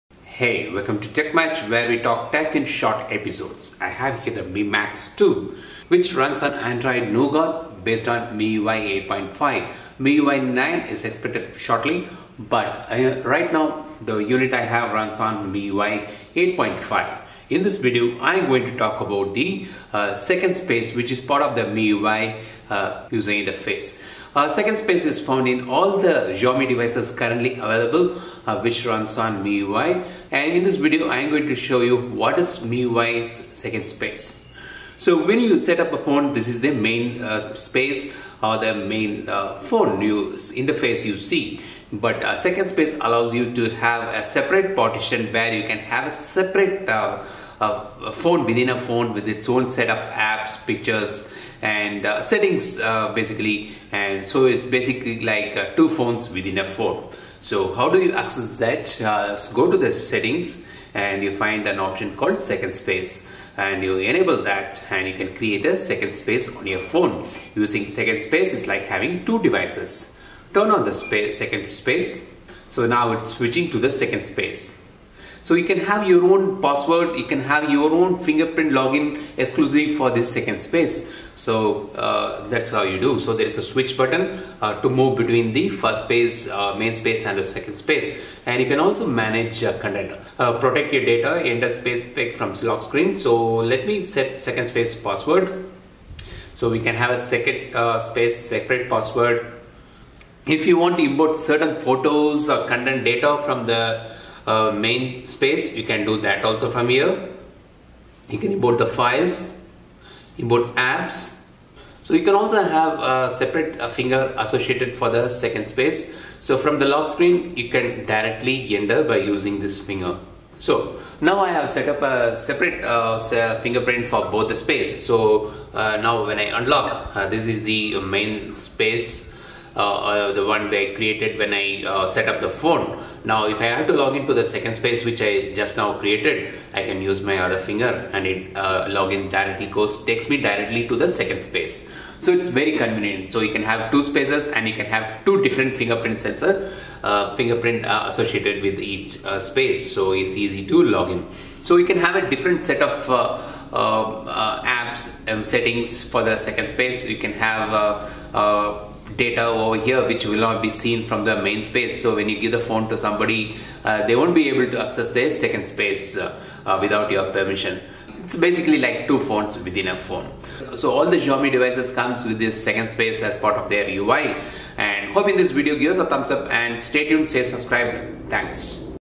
What is Second Space in MIUI? How to Set It up Features in Mi Max 2 | Tech Much
Second Space is an interesting feature in MIUI which enables two phones within a phone. Here is a quick demo. It is also available in other Xiaomi devices running on MIUI 8 and above.